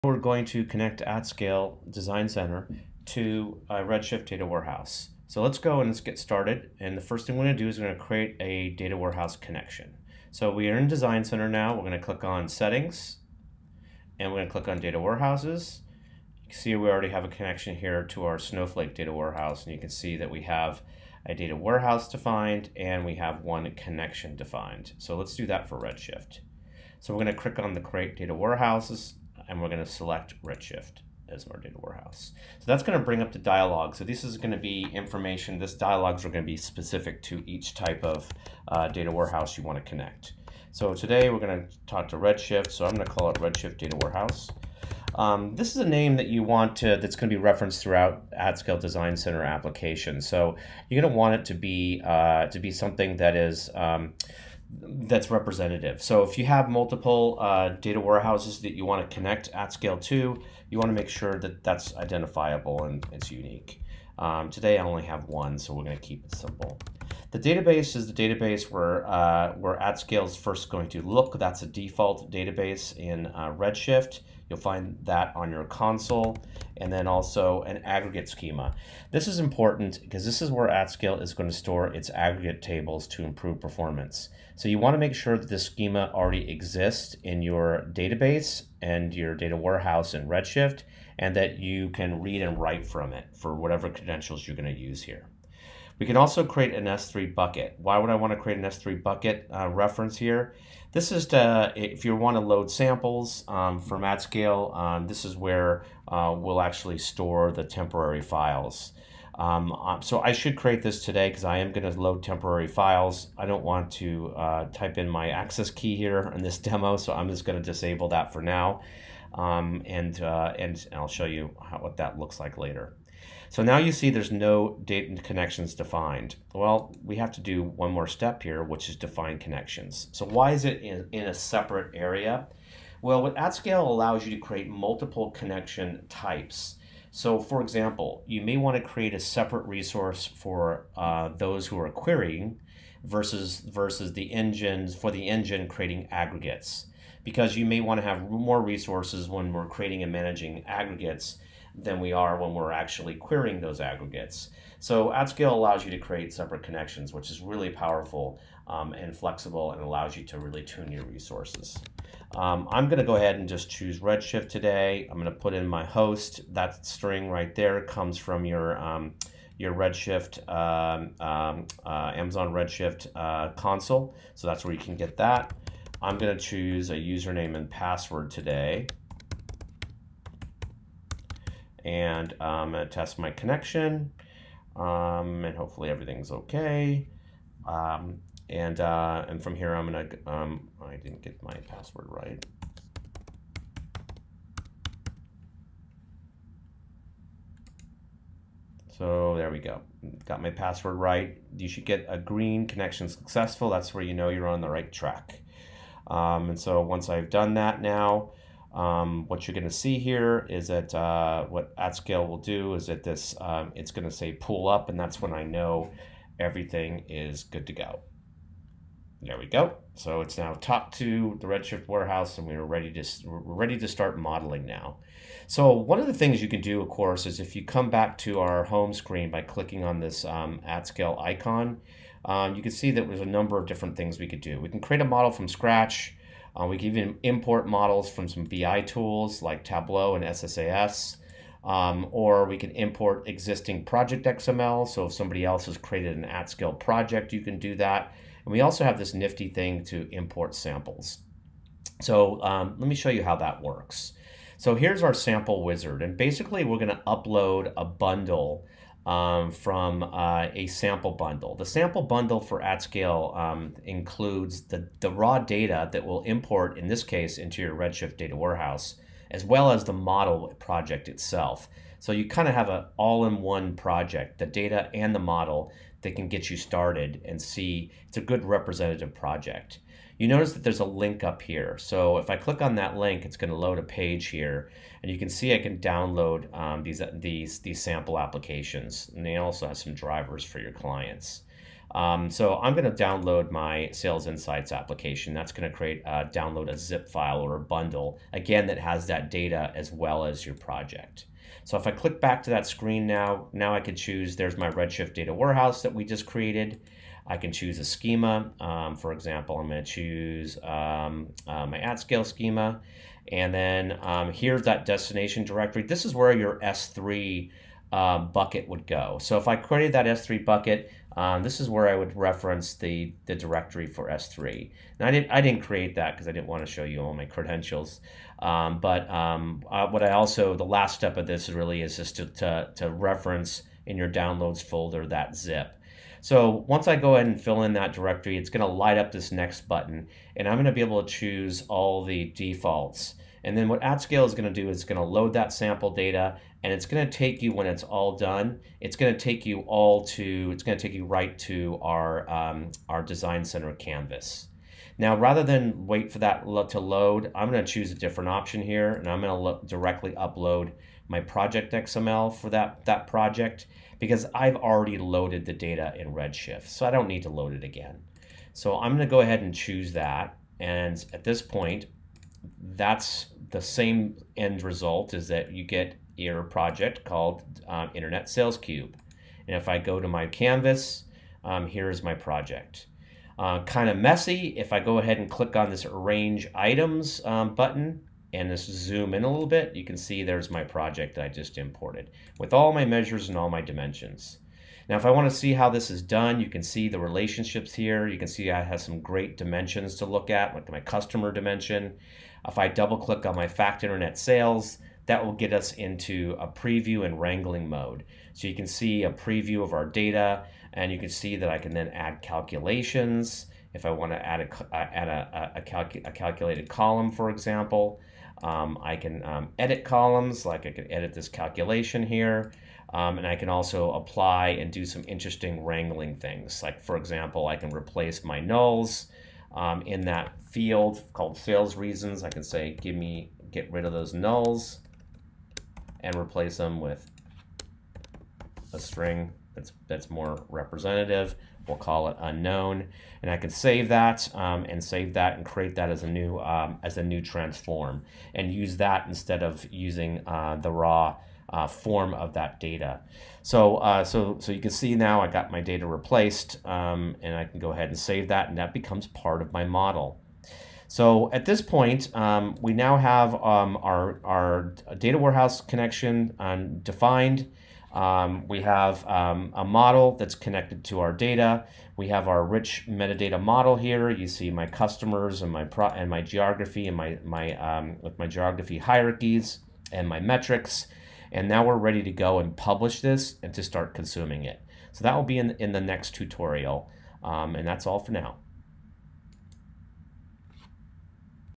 How AtScale’s Semantic Layer Works With RedShift
Learn how AtScale’s Semantic Layer works on RedShift as a Cloud Data Warehouse. Watch this video to learn how to connect AtScale Design Center to Redshift Data Warehouse with the following two steps:

1. Create a Data Warehouse Connection with Redshift
2. Understand the various ways to import and begin modeling your project

You will also see a use case with modeling Sales data and adding dimensions to make more strategic decisions based on relevant OKRs and KPIs in tools like Power BI and Excel.

Take your company to the next level and make smarter, data-driven decisions with AtScale.